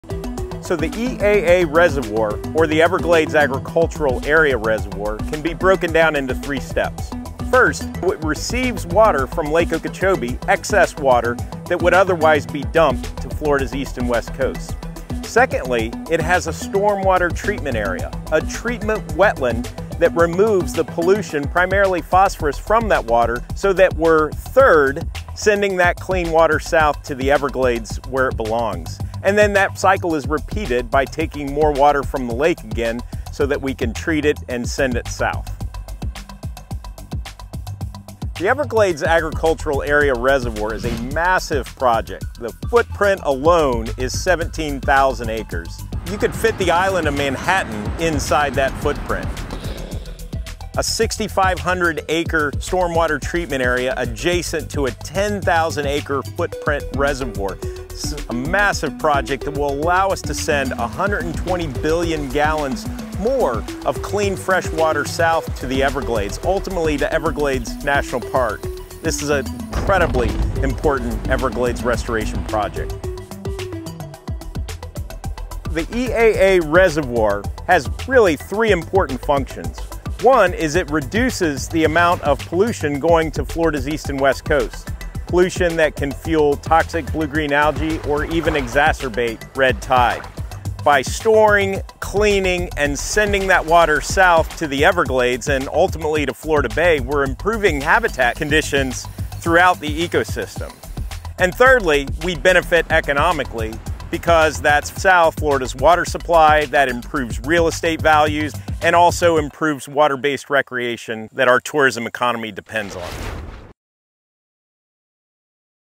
The Everglades Agricultural Area Reservoir
The EAA Reservoir will be located south of Lake Okeechobee and is designed to store excess Lake Okeechobee water so it can be purified in an adjacent stormwater treatment area (STA) and then sent south to nurture the Everglades and Florida Bay. The reservoir will also supply clean freshwater to millions in South Florida and help reduce harmful discharges of polluted Lake Okeechobee water to Florida’s east and west coasts.

The Everglades Foundation Chief Science Officer Dr. Steve Davis answers questions about the EAA Reservoir: What is it? How big is it? What are its functions?